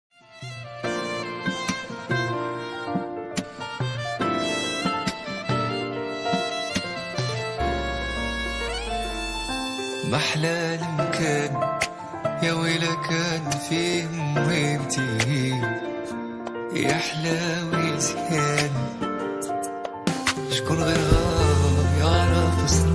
🌹Nezha Regragui 💕actrice marocaine
Nezha Regragui ( born December 17, 1957) is a Moroccan theatre, TV and film actress. Regragui has participated in several plays and films, including Goodbye Mothers. She is also notable for being married to the singer Bachir Abdou and both being the parents of the Moroccan singer Saad Lamjarred.